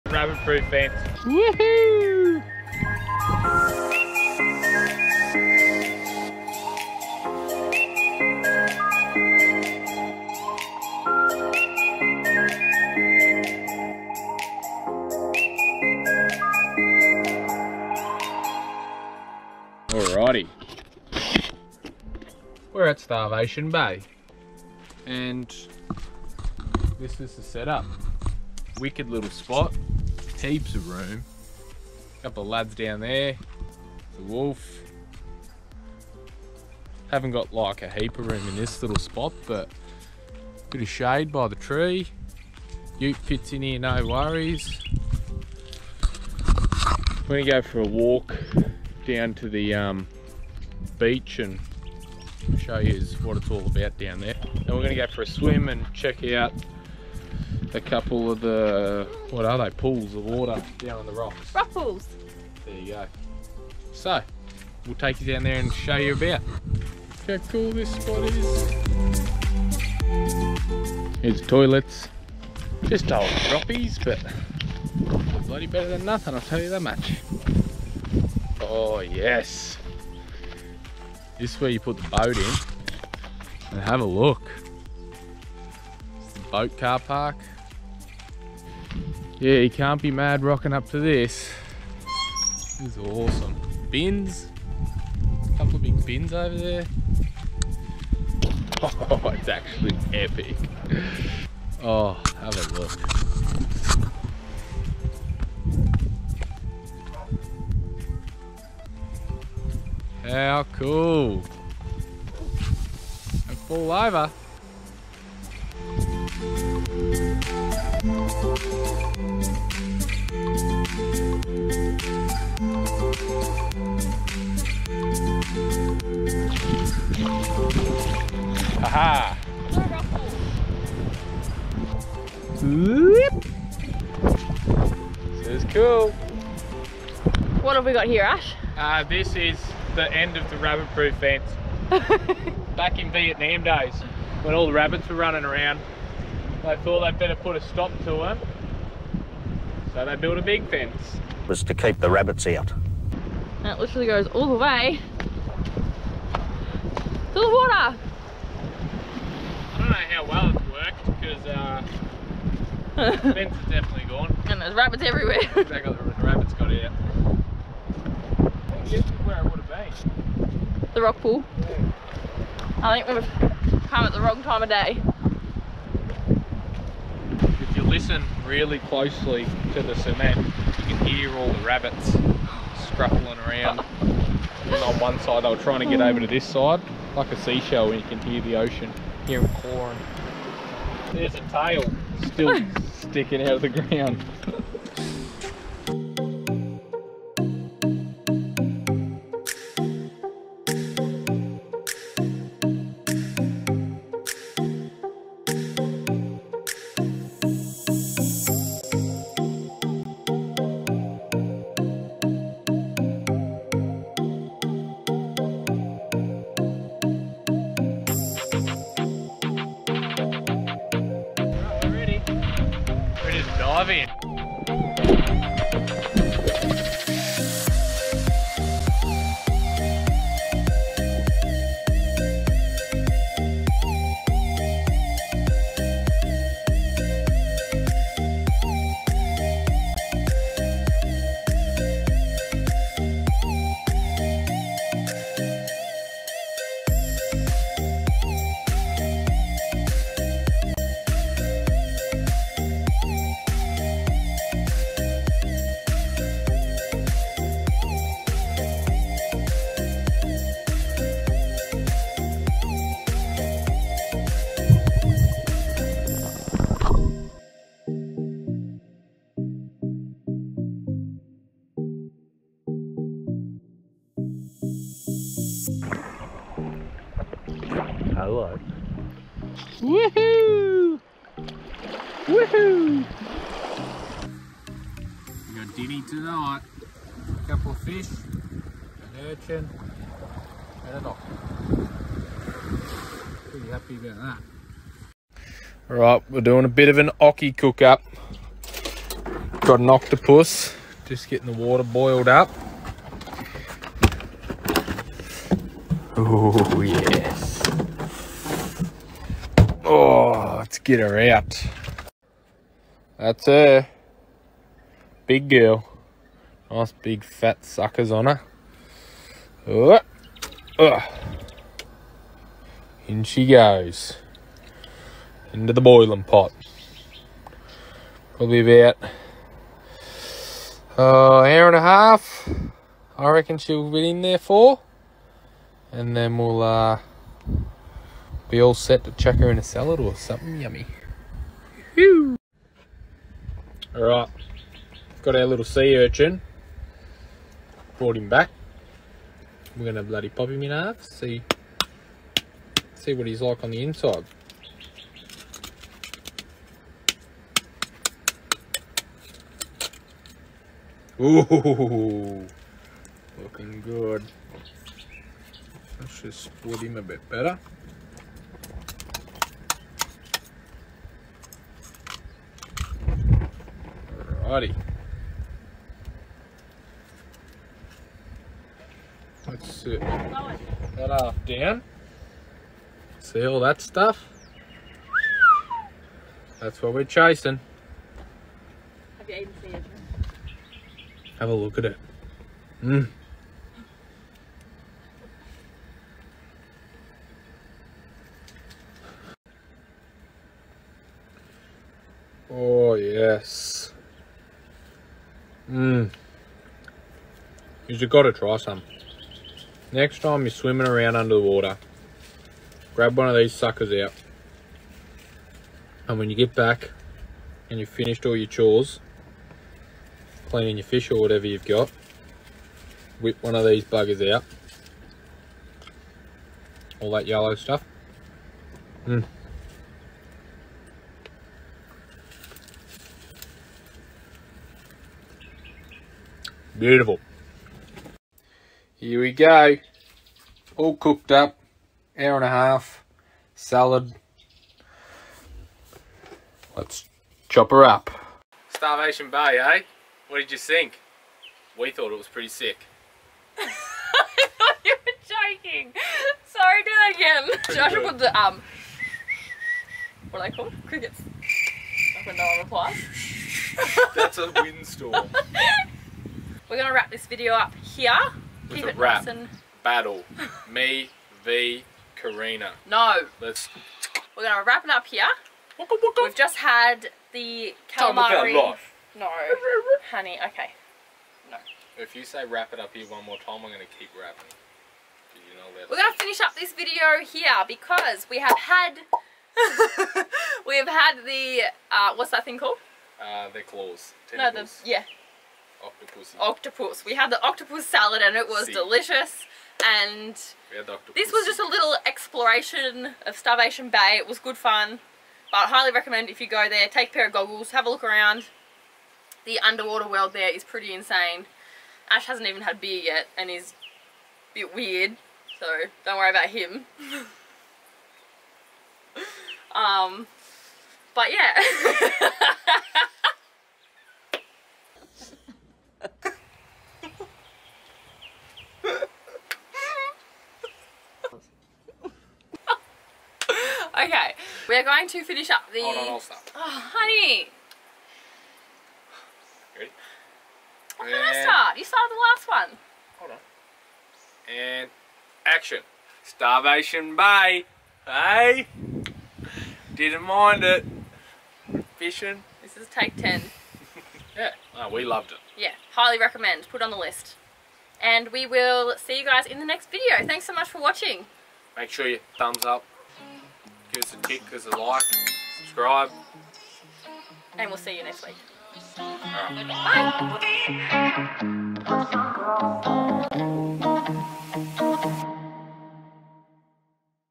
Starvation Bay Exploration! Catch & Cook Octopus & Sea Urchin!!
We explore the beach of Starvation Bay, chasing rock pools and other cool sites including an old rabbit proof fence! Ash goes diving and brings home the goods with some fish, a sea urchin and an octopus! Safe to say we never go hungry! After a day adventuring Starvation Bay on and off land, Ash cooks up the octopus to add to a yummy salad.

Ep. 2